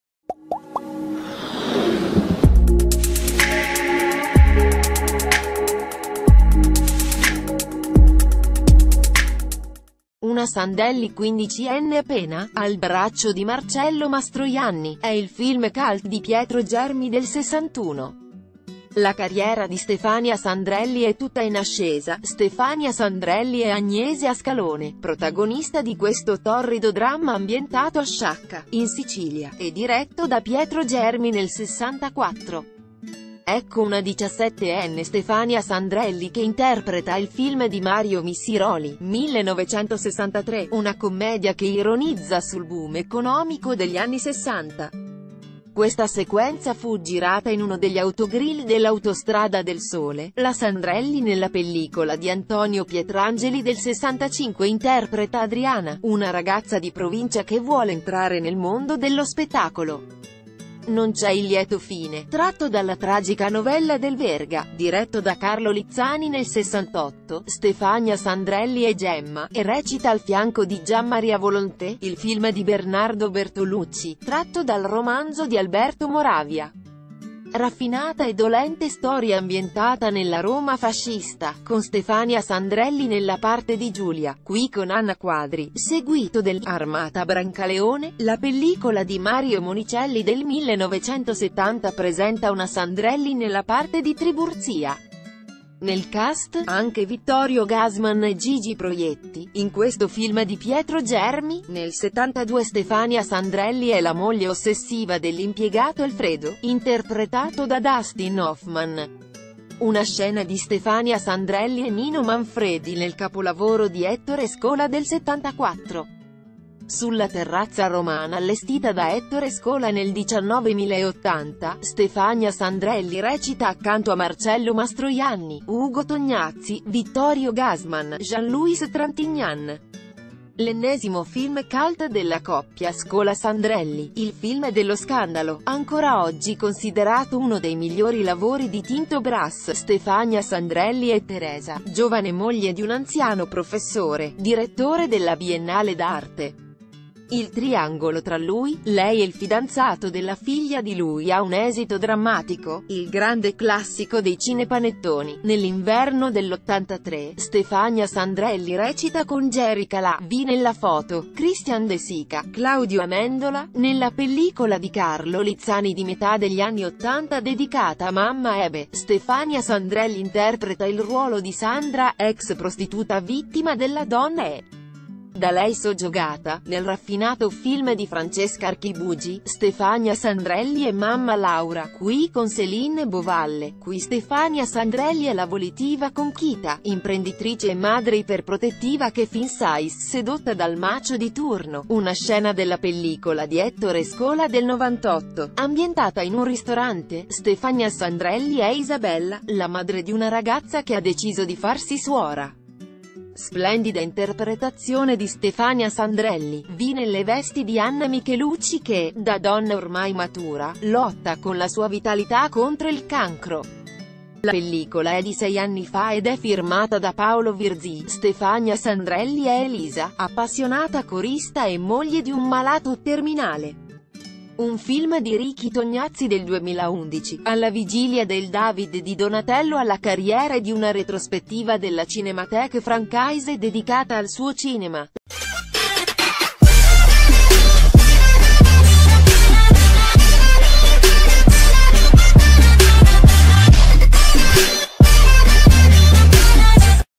Stefania Sandrelli: «Sono tutta istinto»
Stefania Sandrelli: «Sono tutta istinto» . Una Sandelli 15enne appena, al braccio di Marcello Mastroianni: è il film cult di Pietro Germi del '61. La carriera di Stefania Sandrelli è tutta in ascesa.  Stefania Sandrelli è Agnese Ascalone, prot... ...